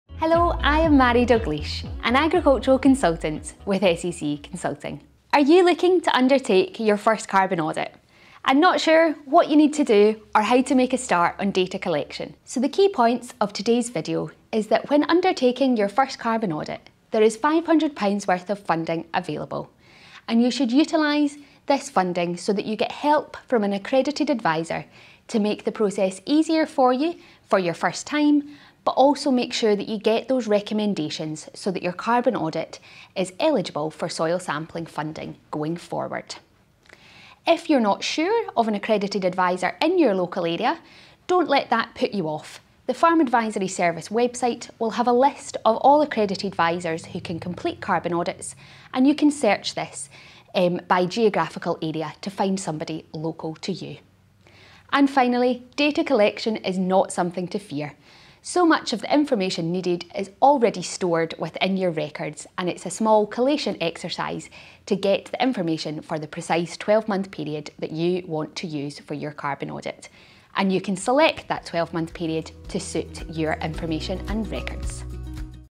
Getting Started with Carbon Audits
Carbon audits are an excellent tool to measure business efficiency and performance. Are you looking to complete your first carbon audit, but not sure where to start or what to do?

Don’t forget there are still grants available, through Preparing for Sustainable Farming, to assist you with carbon audits and soil analysis. If you act now, while support is available, you will be prepared for 2025.

Scotland’s Farm Advisory Service (FAS) is funded by the Scottish Government, providing information and resources aimed at increasing the profitability and sustainability of farms and crofts.